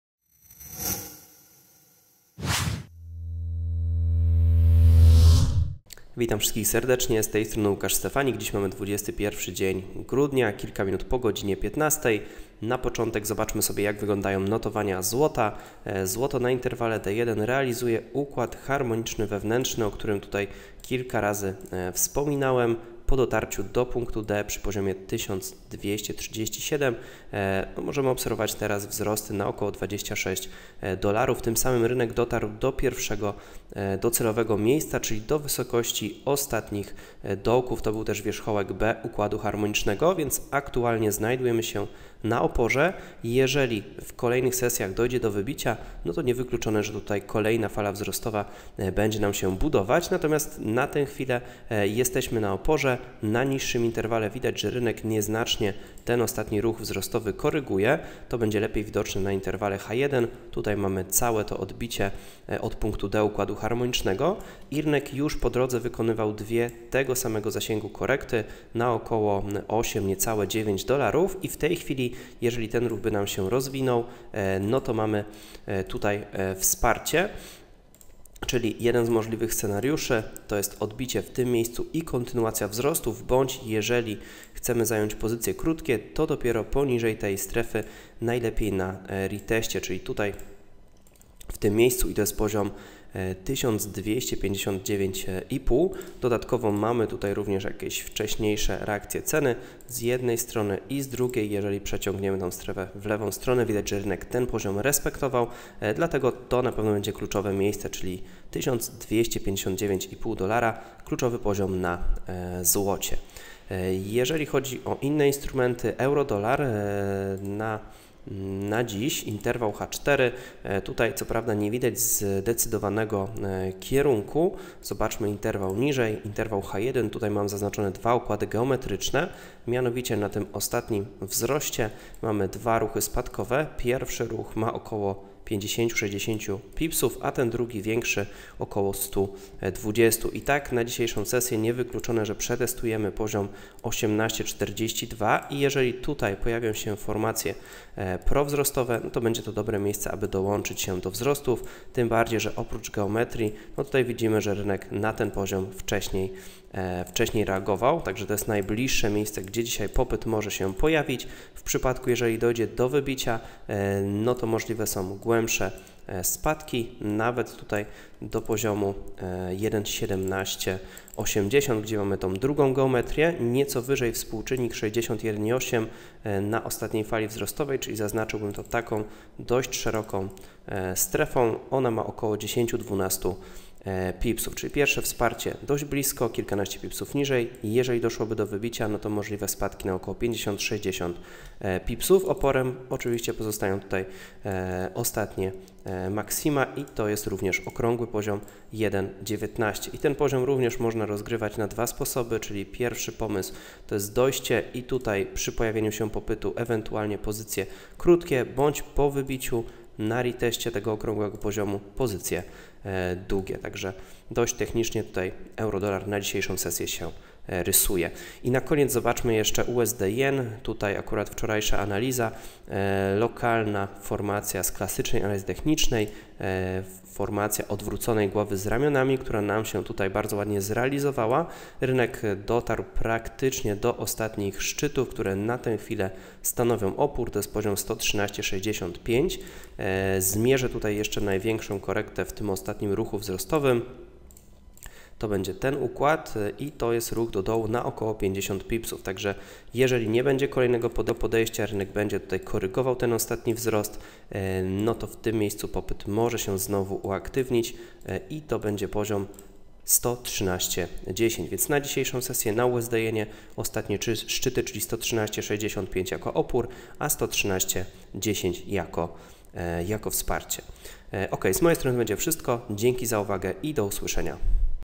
Szybka analiza - EUR/USD, USD/JPY, XAU/USD | 21 grudnia
Łukasz Stefanik przeprowadza analizę harmoniczną video EUR/USD, USD/JPY, XAU/USD na dzień 21 grudnia 2017.